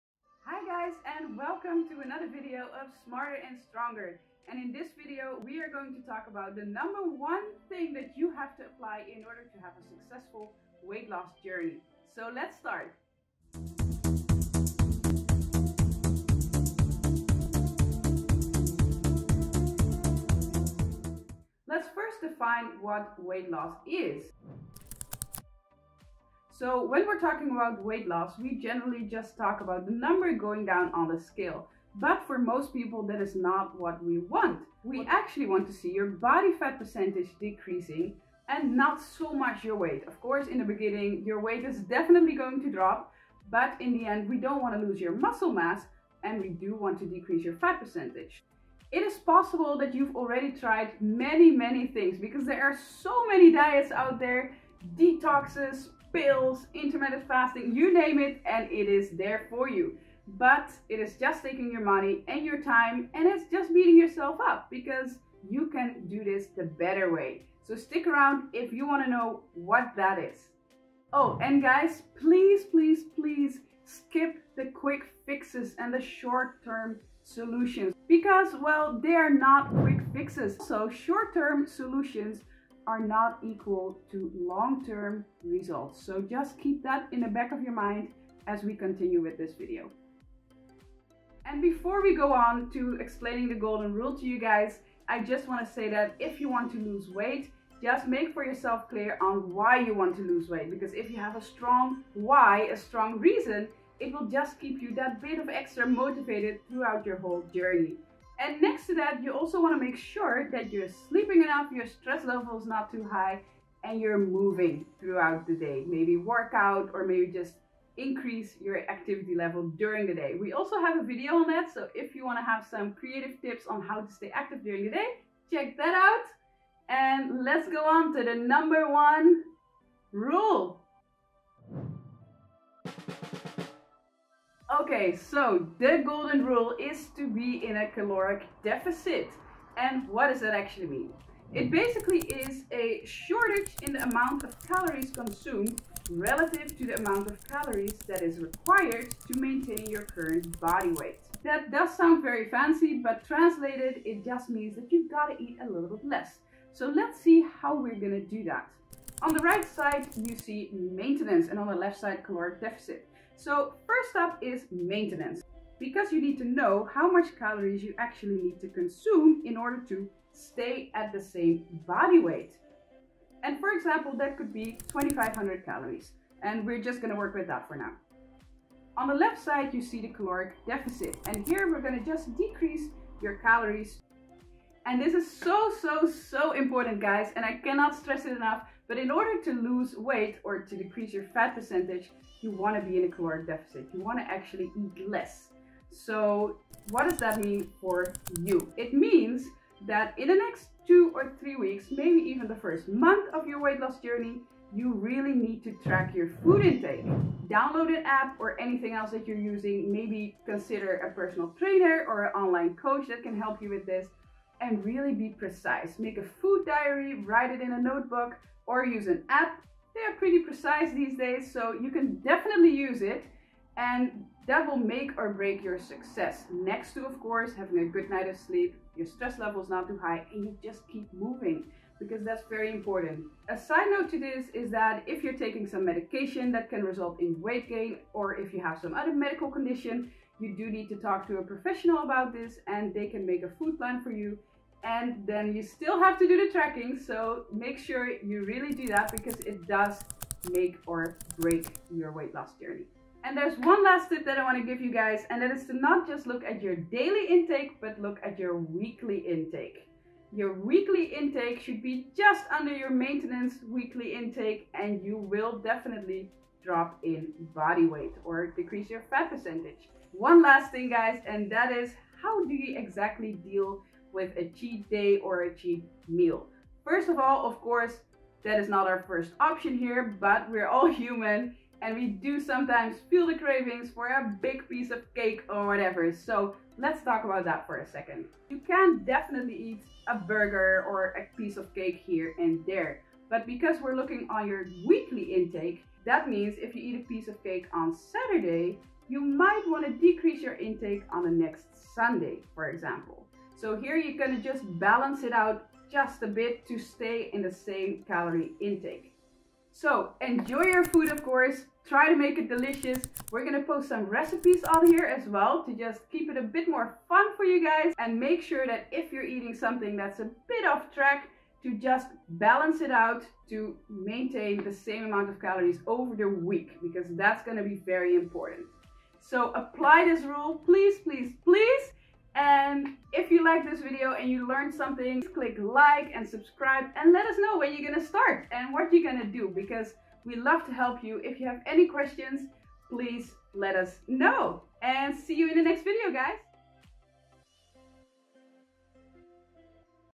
How To Really Lose Weight | THE Video For Weight Loss
Hi guys!

A new video for you on 'How To Really Lose Weight'! Together we will look at the best way to lose weight and also discuss the topic of cheat meals and cheat days, so get ready!

Let us know what your goal is in the comments!

Stay Smart & Strong!

Much love,
Parwiz & Annemarie